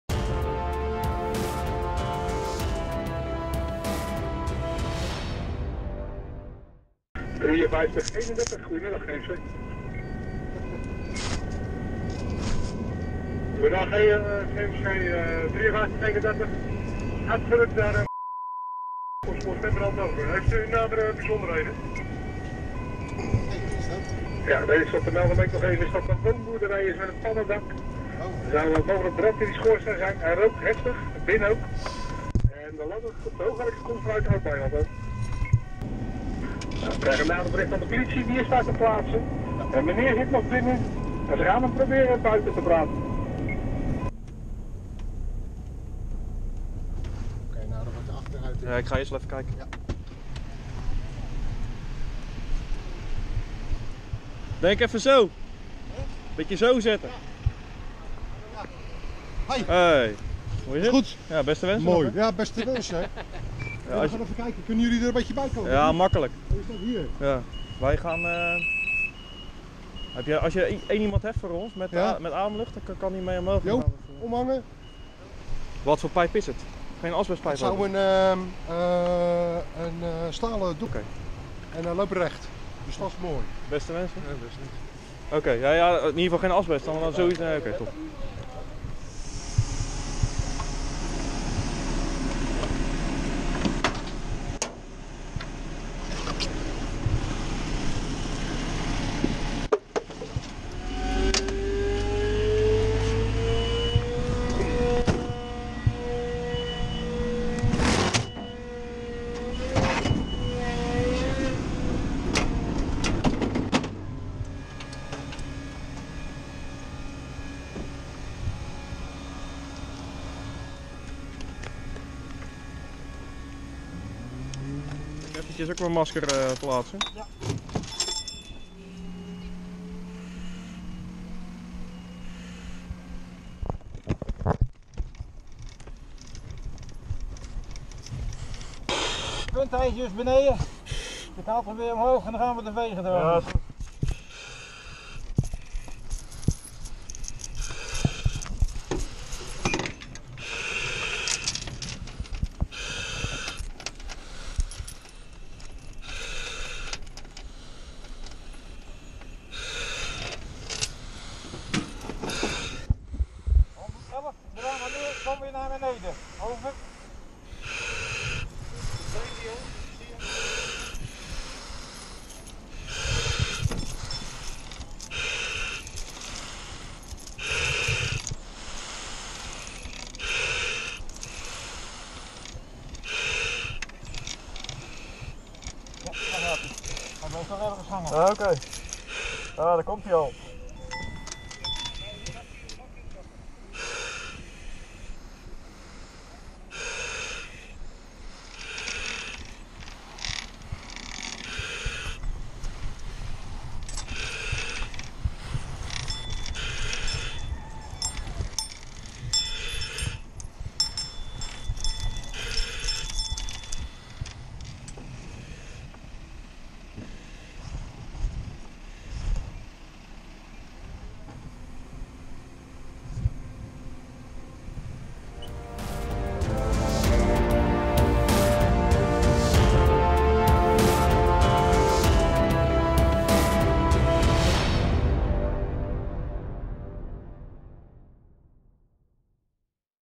Assistentie bij schoorsteenbrand met hoogwerker
VIDEO: Assistentie bij schoorsteenbrand met hoogwerker

Zaterdagmiddag 6 januari is onze hoogwerker uitgerukt naar de buurtgemeente Zuid-Beijerland ter assistentie bij een schoorsteenbrand.

Kijk mee met de bemanning van de HW5451 en krijg een unieke indruk van onze werkzaamheden.